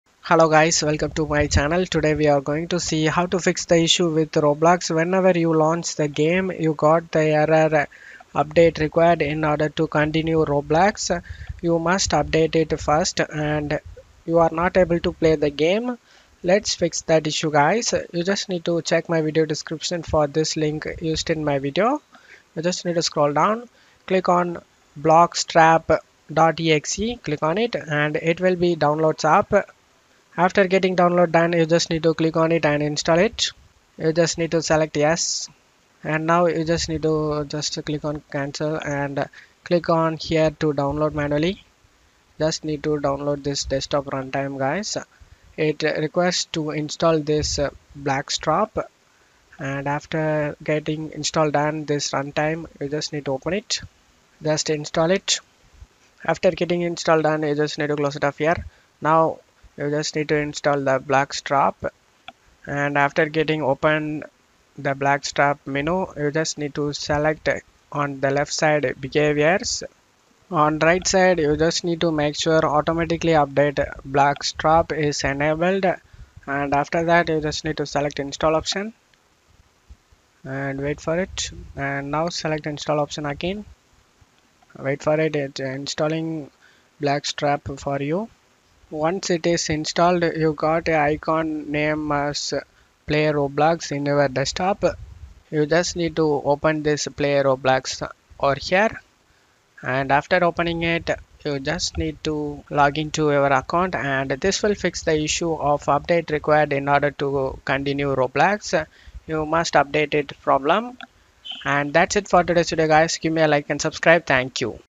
How to Fix Roblox "Update Required" Issue (Guide)
Encountering the "Update Required" problem in Roblox can be frustrating, preventing you from enjoying the latest features and improvements. This guide provides step-by-step solutions to resolve the issue, ensuring a seamless Roblox experience. Stay up-to-date and dive back into the immersive world of Roblox without interruptions!

In This Tutorial :
how to fix roblox update required problem
update required when using the older version of Roblox